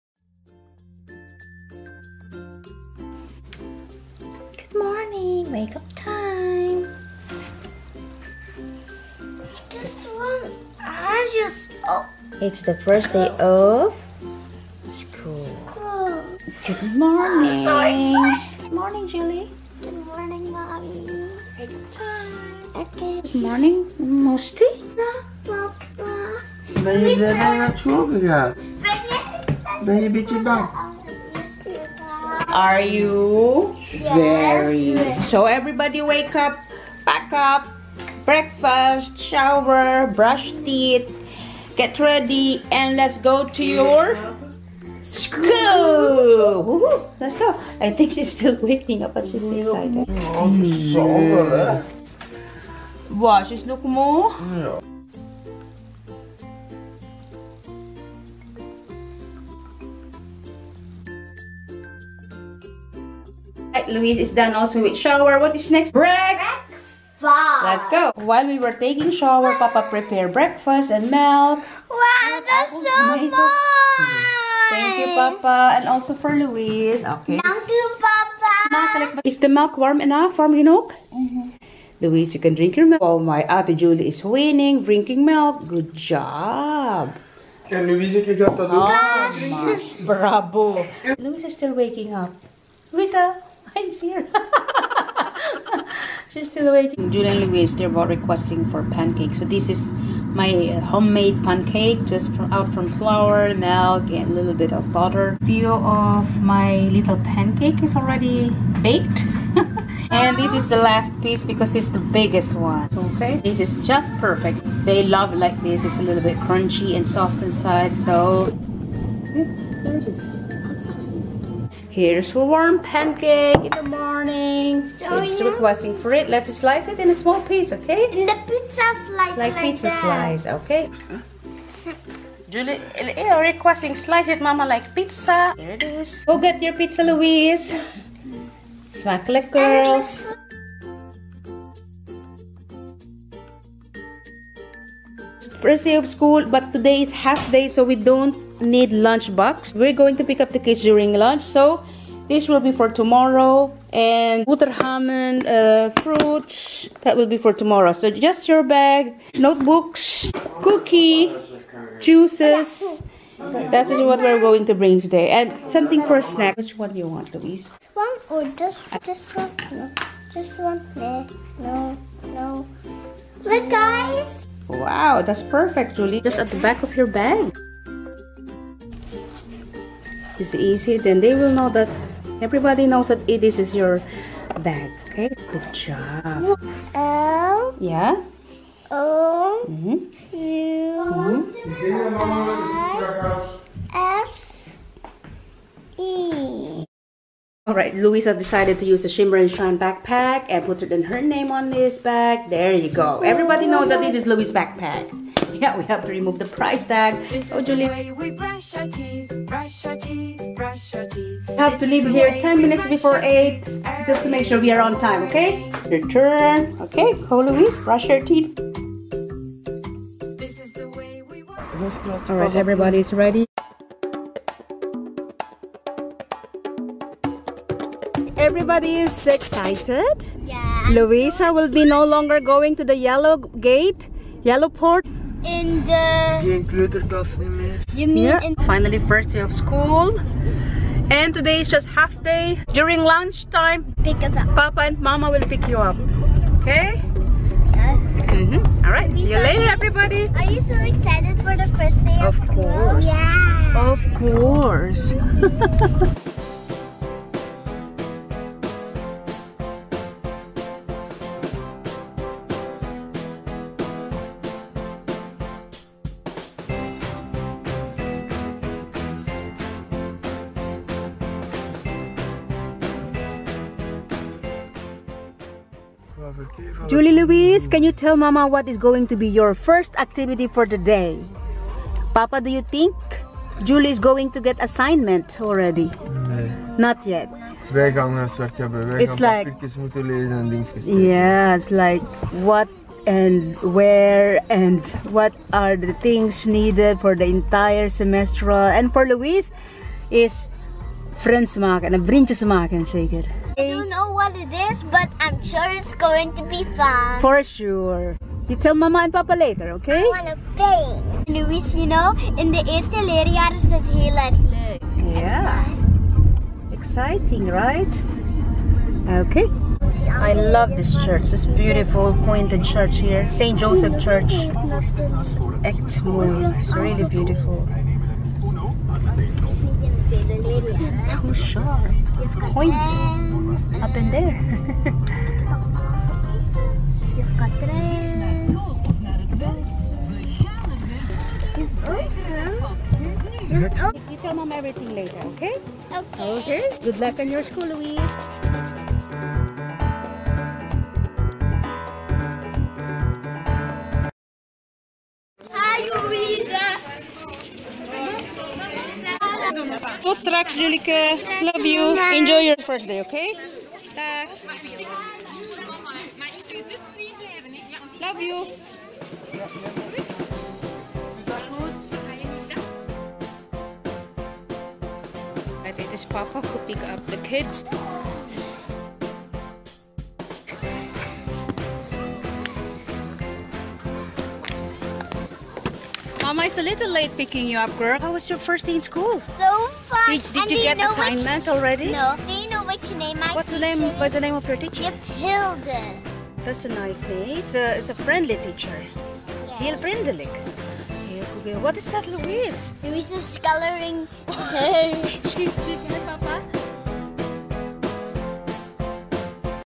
Family Fun Morning Routine - FIRST DAY OF SCHOOL
Our morning routine during First day of school with Family fun  Sept21